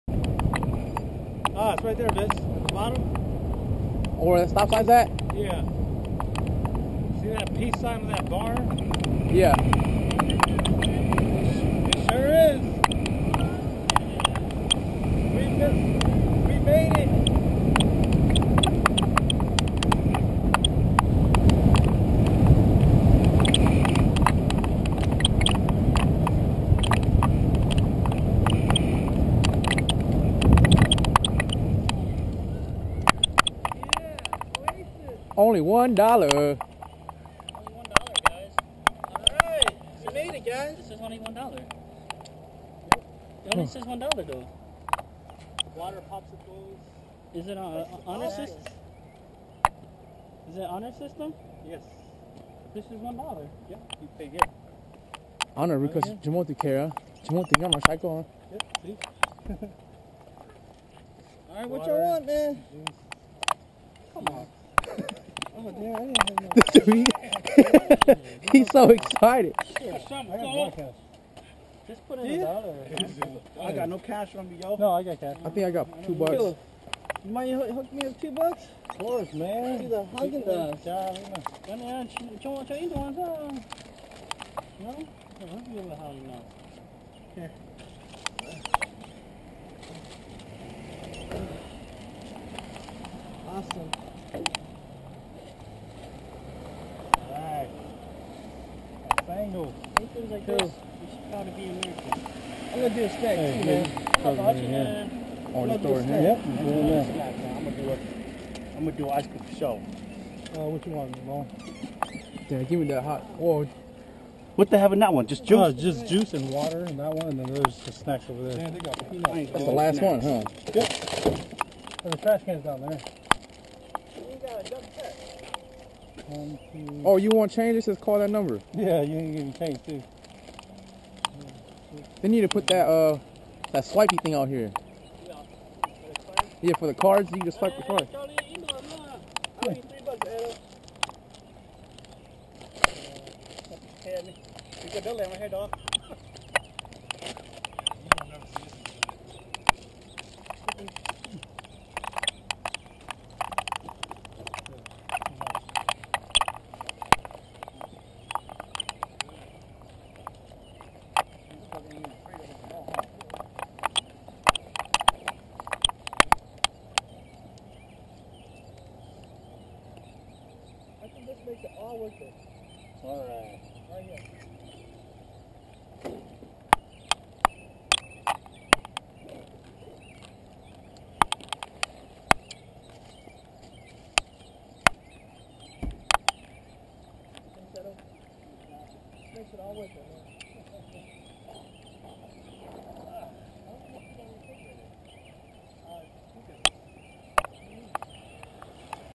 Oasis stand on Akron Ohio Bike & Hike Trail
Footage of a honor system self-serve stand set out by the landowner for bikers and hikers who happen to come by. It's a sweet stop and helped restore some of my hope for humanity.